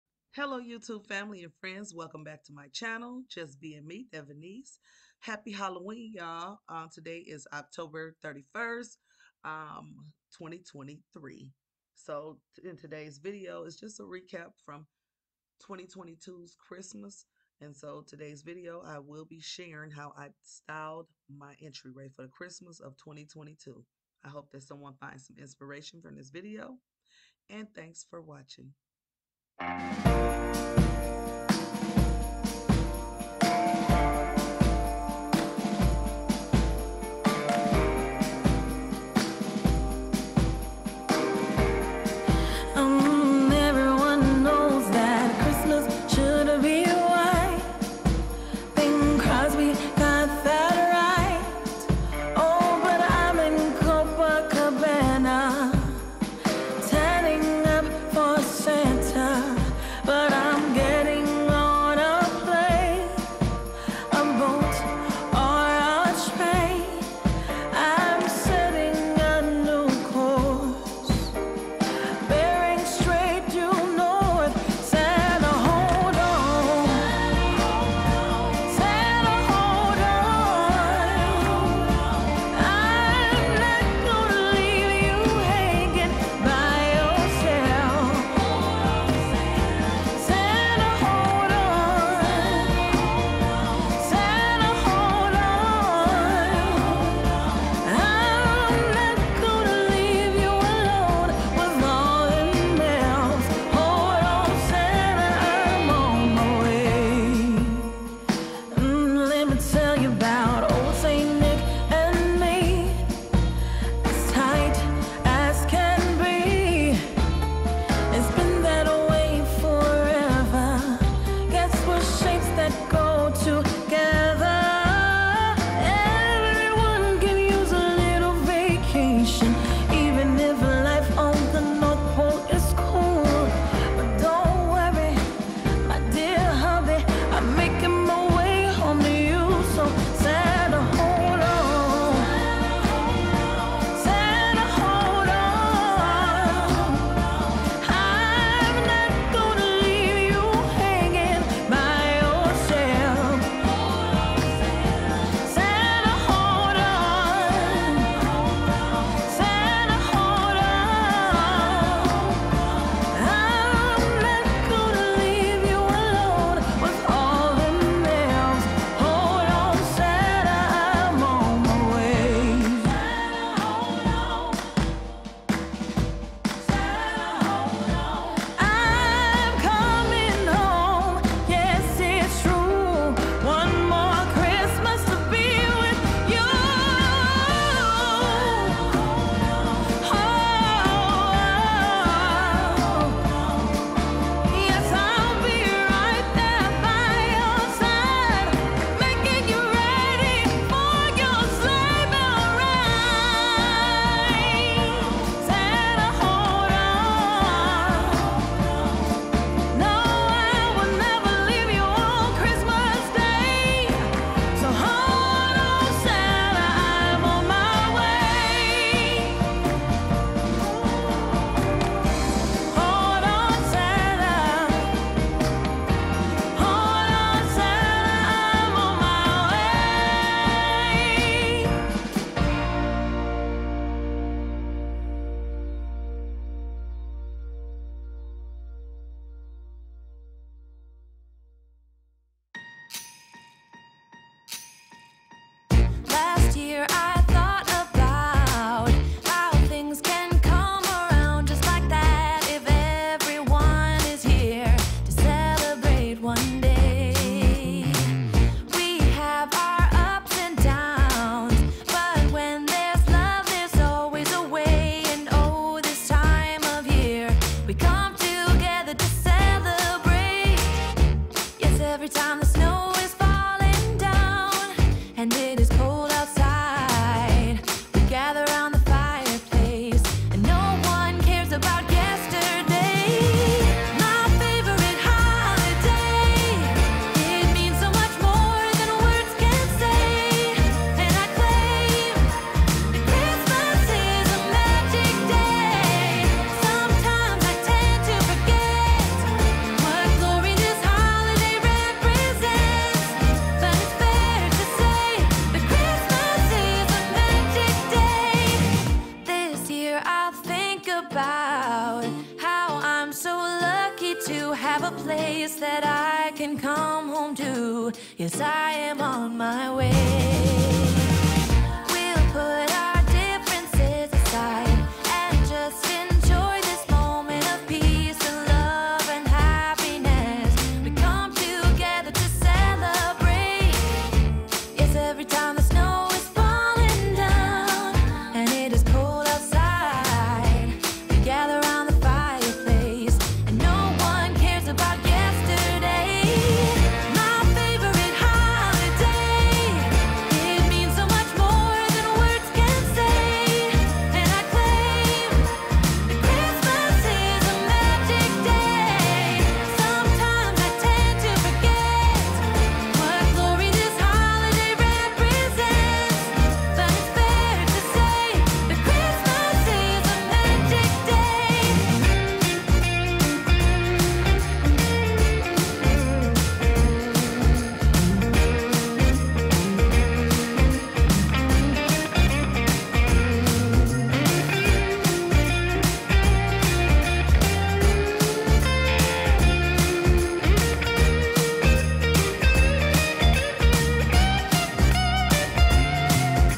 BLACK AND WHITE CHRISTMAS ENTRYWAY DECOR IDEAS throwback #2022
Today's video is a 2022 Christmas throwback video that I hope you find some inspiration from.